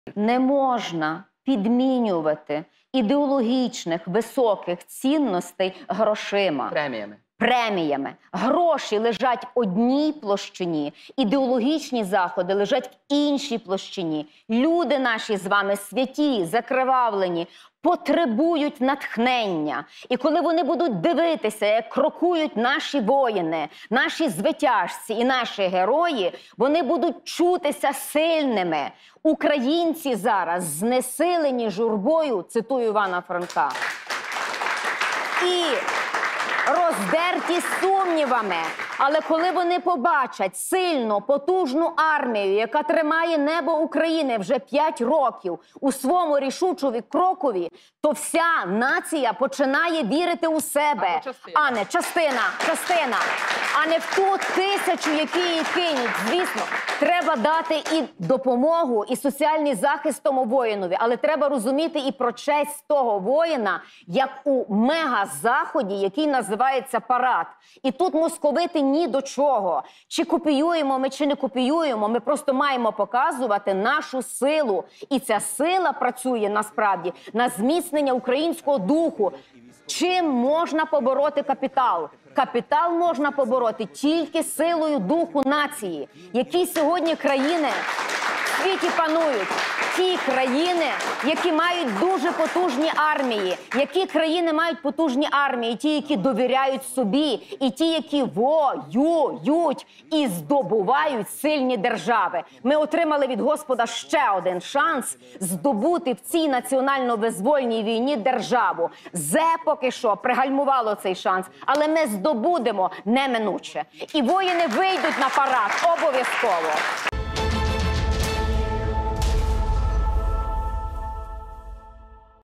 Ірина Фаріон: "Капітал можна побороти тільки силою духу нації."
"Капітал можна побороти тільки силою духу нації. І воїни вийдуть на парад обов'язково!" - Ірина Фаріон